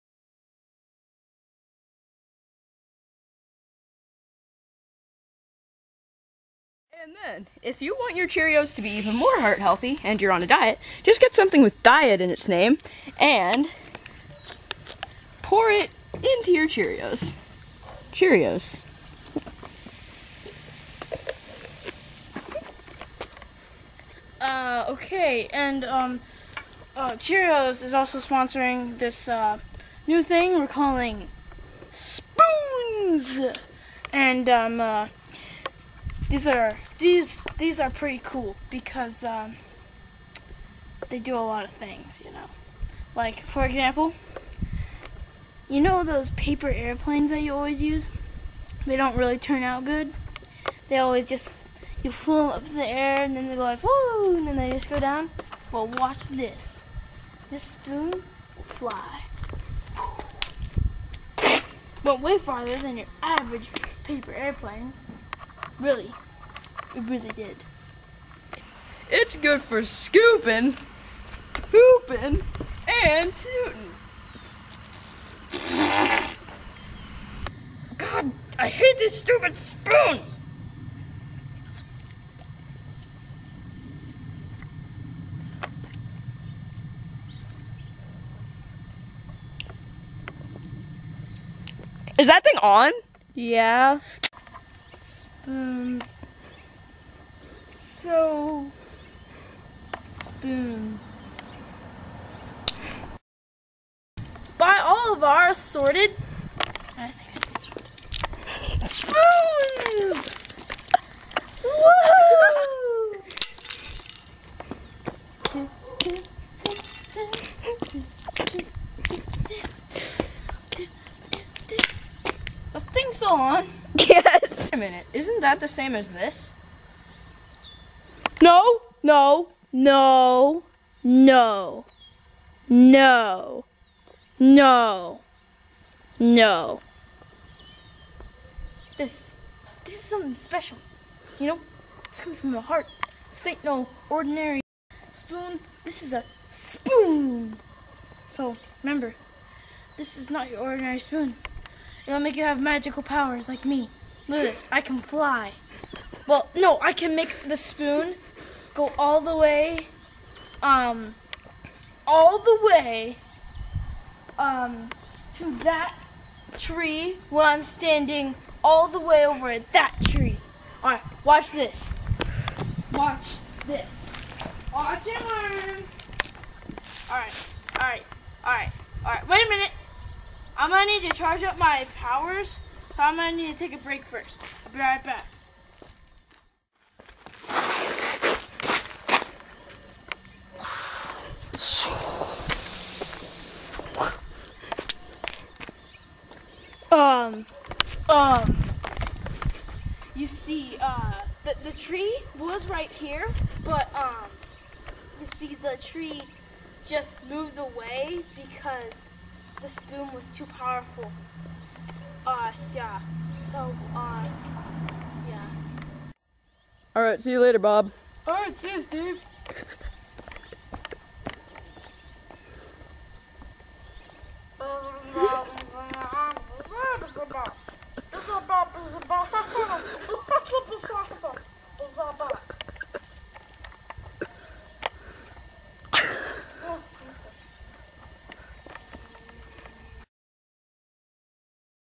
Spoons
In this infomercial, we will be showing the new product, "SPOONS" But are they what the seem?

Recorded Augest